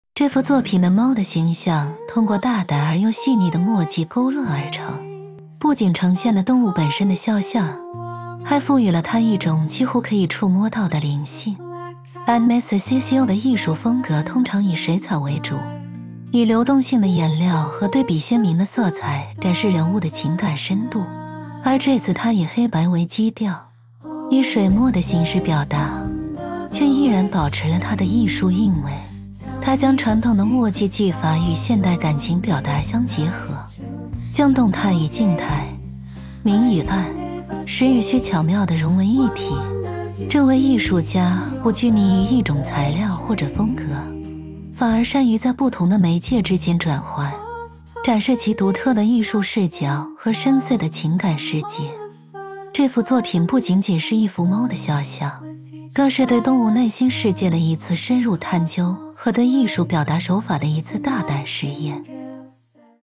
Whispers of Whiskers: Agnes Cecile's Feline Fantasia
In the realm of modern art, where creative expression is boundless, Agnes Cecile emerges as a beacon of innovation and emotional depth. Through a fusion of spontaneous ink splatters and precise strokes, she captures not just the likeness but the essence of her feline subjects. This video unveils the captivating process behind Agnes Cecile's latest masterpiece, "Whispers of Whiskers", a striking portrayal of a cat that seamlessly bridges the gap between abstract art and realism.

Dive into the behind-the-scenes as we explore Agnes Cecile's studio, where her black and white canvas defies the traditional portrayal of cats, often relegated to cute and cozy corners of art. Instead, she portrays a creature both enigmatic and relatable, with eyes that hold galaxies of untold stories. Her technique is a dance between control and chaos, as she allows the ink to find its path, creating textures and contrasts that evoke a sense of wonder and introspection.

As a narrative-driven artist, Agnes Cecile's work is a visual symphony that speaks to the soul. Each painting tells a tale, transcending cultural barriers and forming an unspoken connection between humans and animals. This video is more than just a showcase of artistic talent; it is an invitation to perceive the world through her eyes—a world where every splash of ink and nuanced line weaves a larger story of life and its fleeting beauty.

Join us on this artistic journey with Agnes Cecile and discover how every whisker, shadow, and gaze in her art is a whisper of the profound relationship we share with our feline companions.